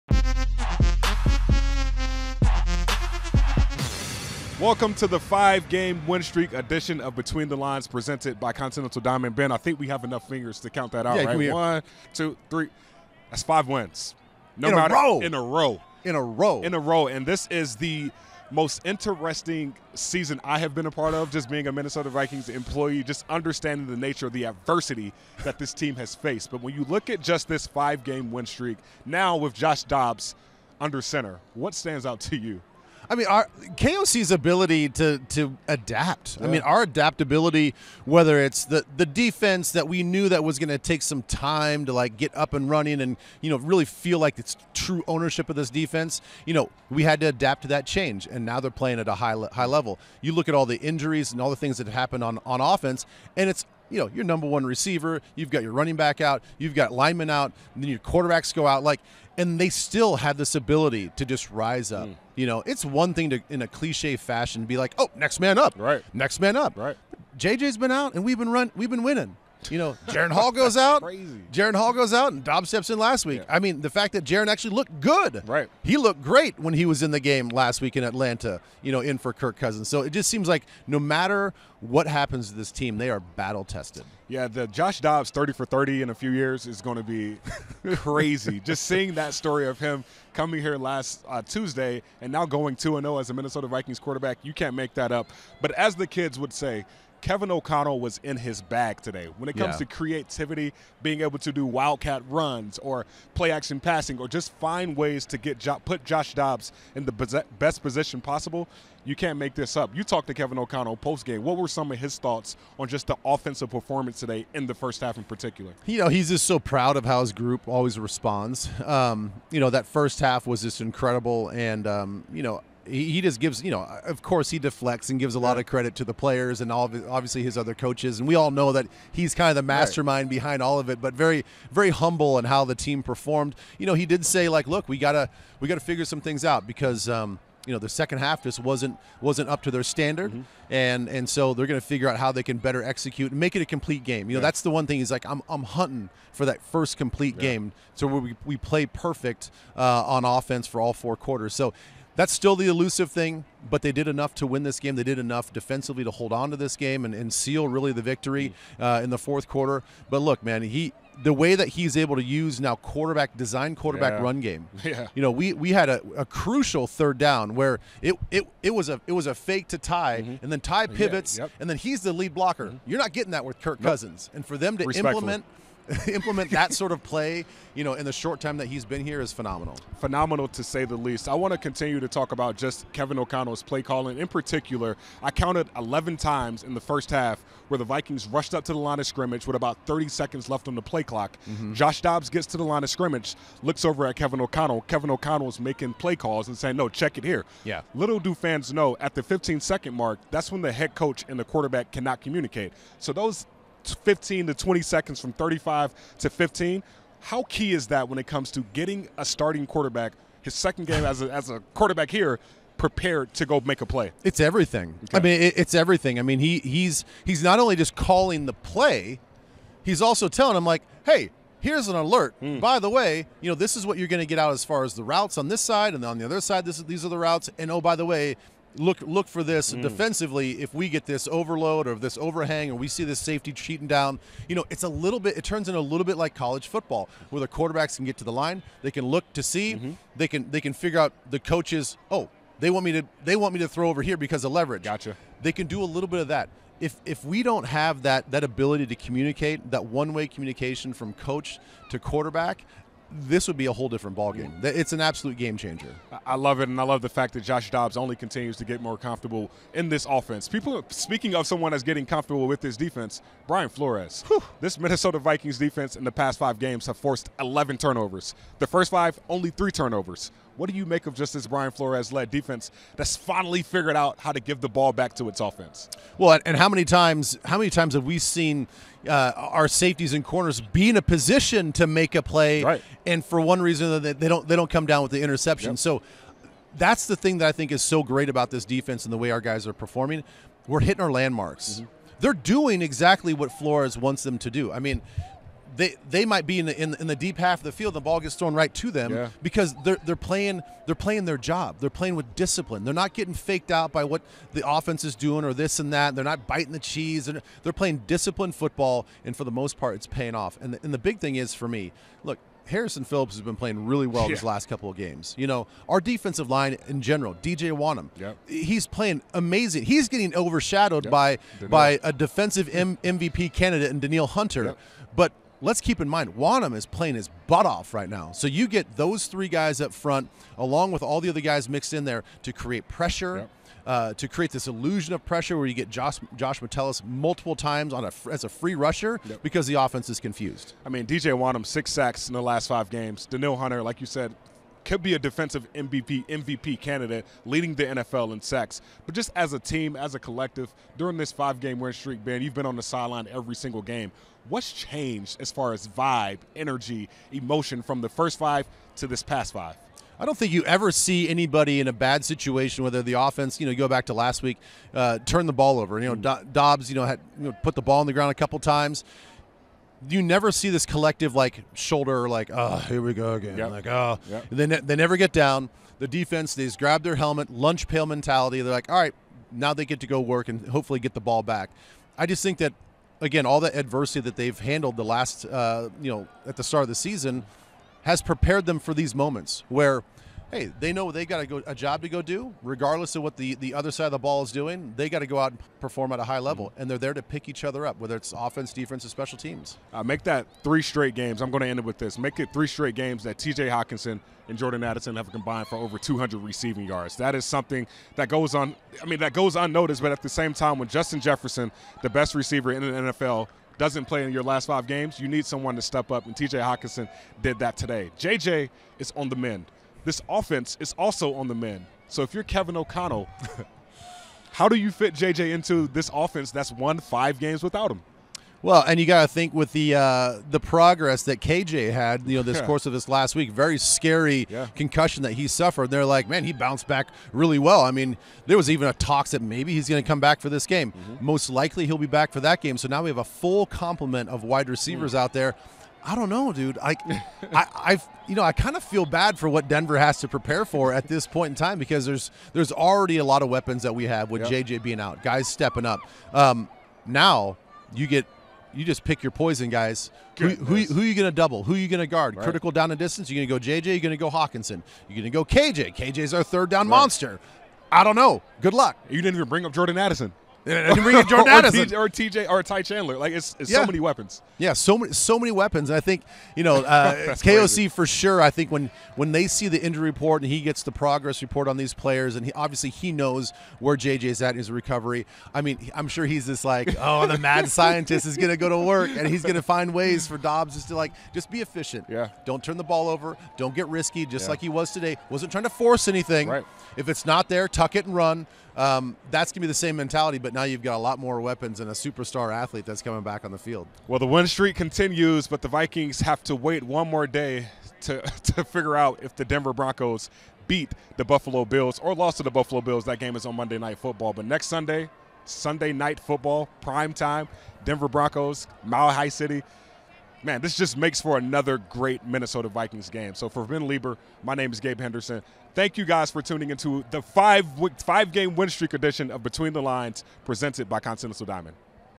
Minnesota Vikings 27, New Orleans Saints 19 | Between the Lines
Vikings Entertainment Network's Gabe Henderson was joined by Ben Leber to break down the Minnesota Vikings 27-19 win over the New Orleans Saints in Week 10 of the 2023 NFL regular season.